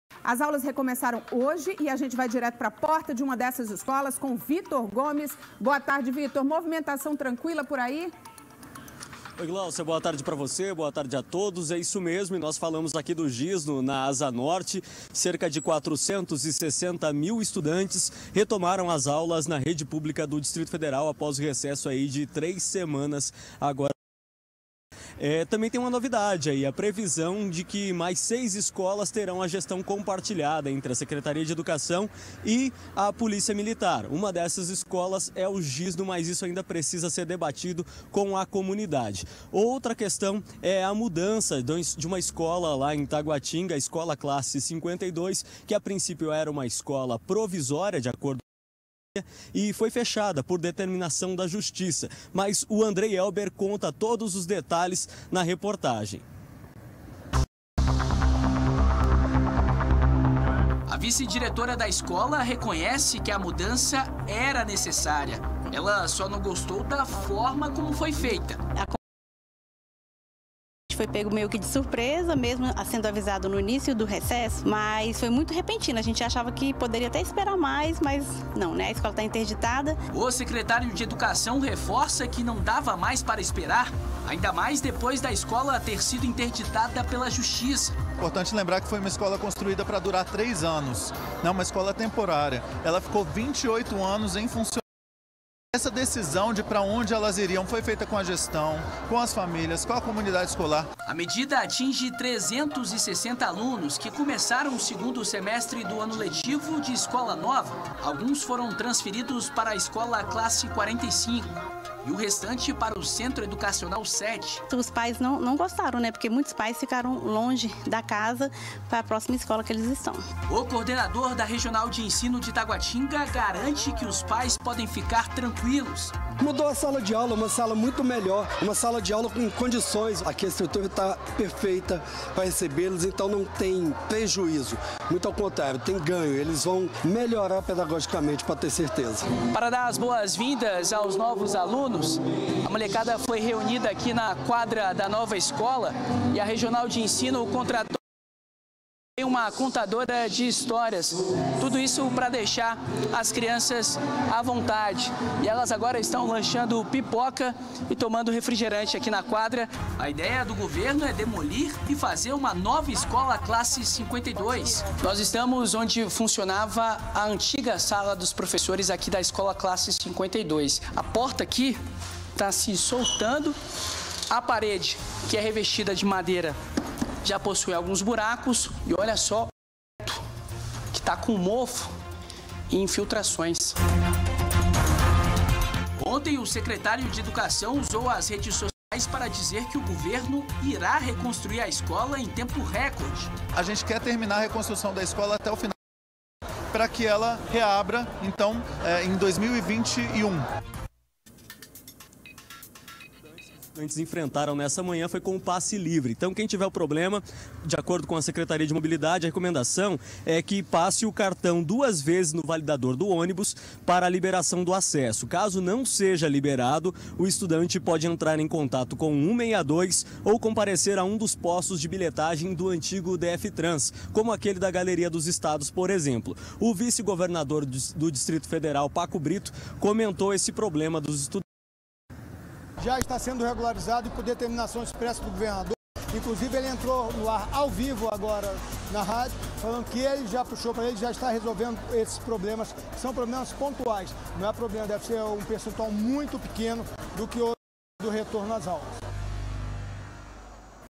JL - Semestre começa com problemas nas escolas públicas
29/07 - Alunos de escola pública realocados por falta de estrutura.